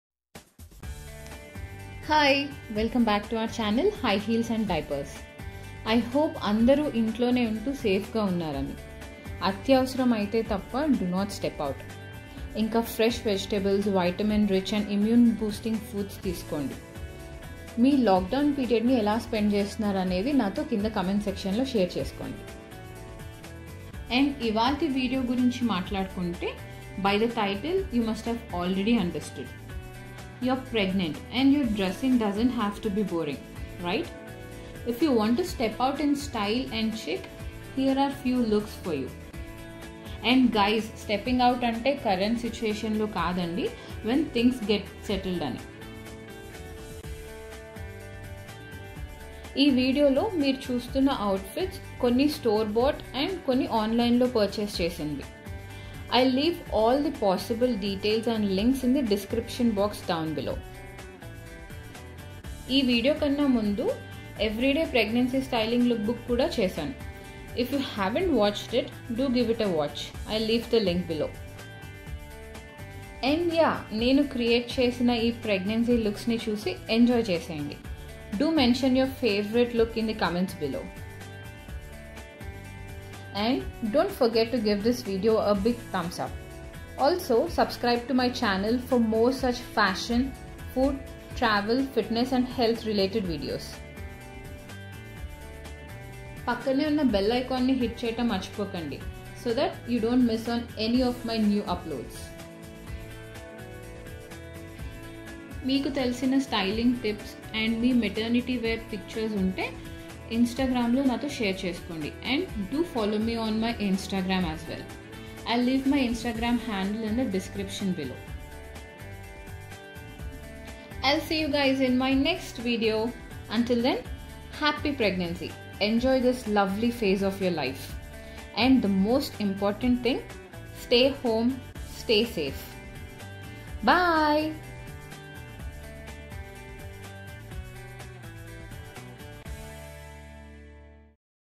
Stylish and Chic Maternity Lookbook // Pregnancy style Part-2 // Fashionable Maternity Outfit Ideas
Hi Guys

Outfit Details :

Look-1

Dress - Only By Vero moda
Footwear - Paprika(lifestyle)
Watch - Guess
All are store bought.

Look-2

Dress - Globus
Footwear -Being Traditional(Instagram Store)
Watch -Guess

Look-3

Dress - Shein
Shrug - Aliexpress
Footwear - Catwalk (lifestyle)
Links are currently unavailable

Look-4

Dress - Globus (store)
Jacket - Amazon.in
Footwear - Amazon.in

Look-5

Dress - Shein
Footwear - Amazon.in

Maternity Lookbook Part-1

A big thank you for everyone who already did🙏

Love&Gratitude,
Haritha Kurmana